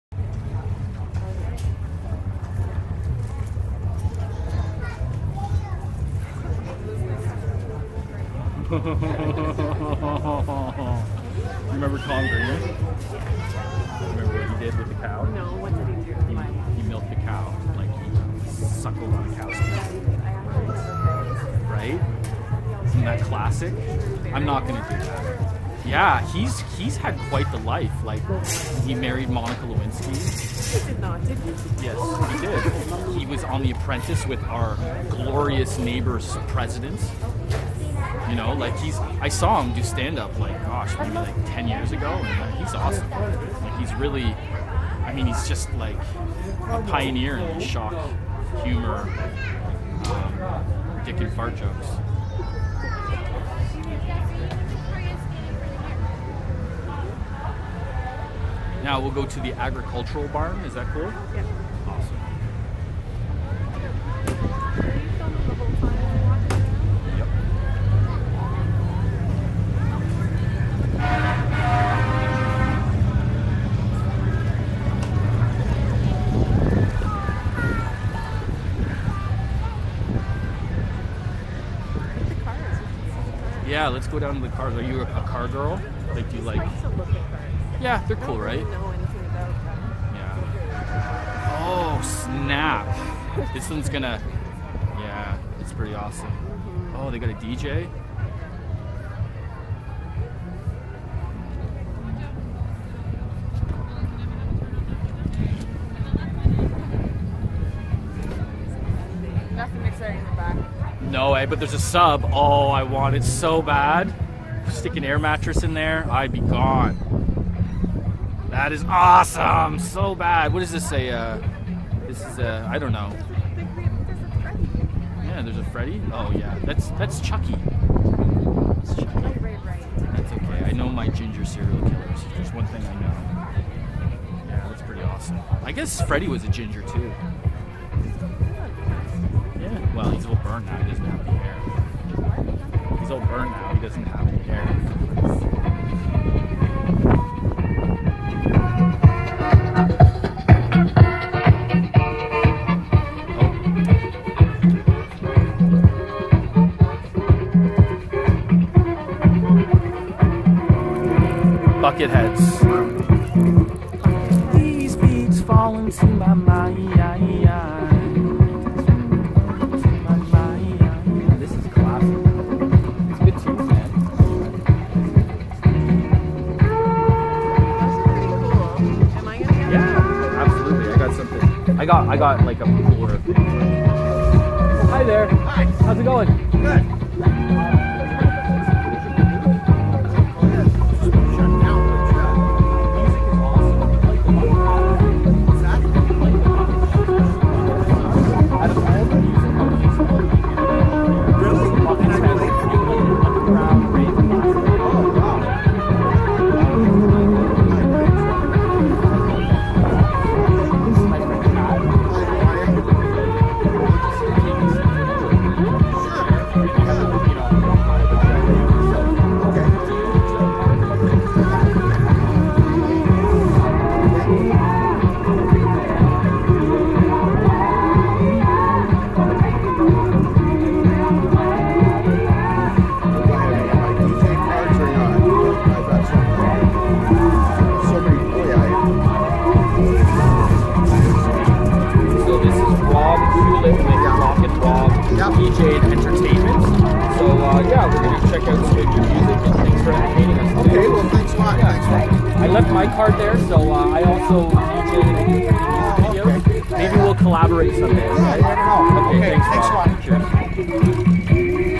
Milton Fair 2019 - 360Video Part 3
The Halton Agricultural Society was formed in March 1853 with Thomas Douglas as the first President. Fall Fairs were then held in the 4 townships of Esquesing, Nassagaweya, Trafalgar and Nelson, until a larger County Fair was established.

In 1959, the current Grandstand was built, with a capacity of 750. Horse racing dominated the entertainment lineup.

Today the main attractions include the demolitionderbies, truck & tractor pulls, lawn mower racing as well as several horse shows. The Milton Fair is a 3-day event and continues to be staged on the weekend of the 4th Saturday in September.